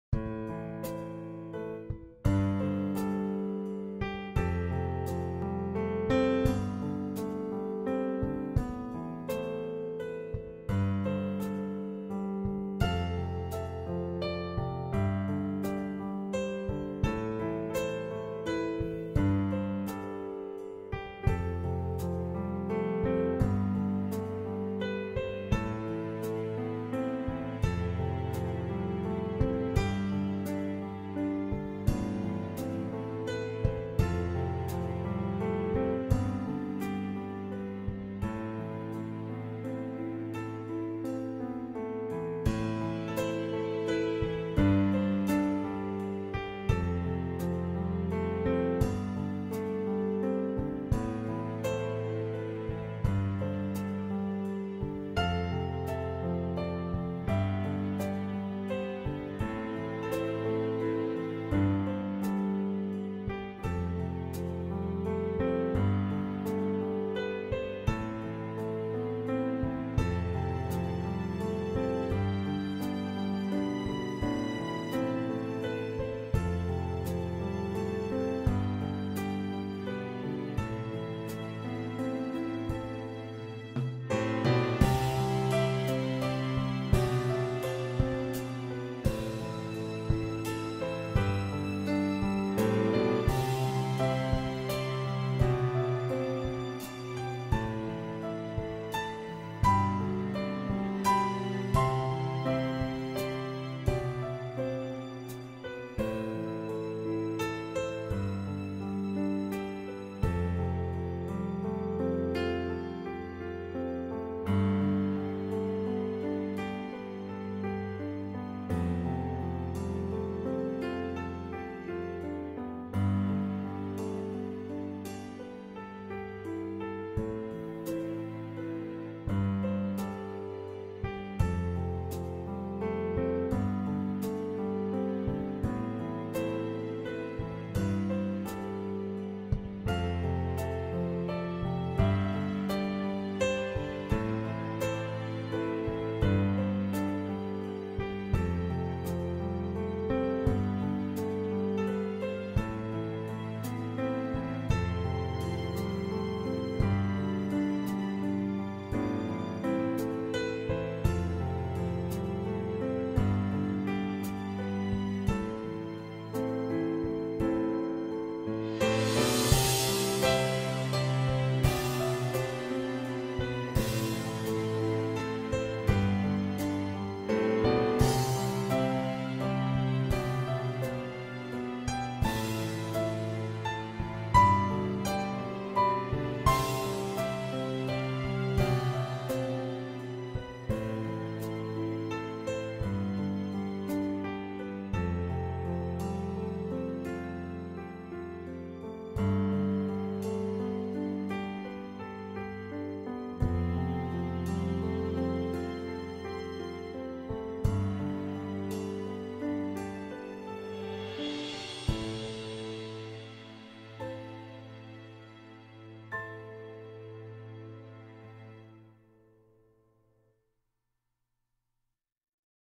Sad story of Oiled Birds
When people see the images in this video, they begin to understand how devastating oil spills are.  The video has been updated since the original was posted, with new music.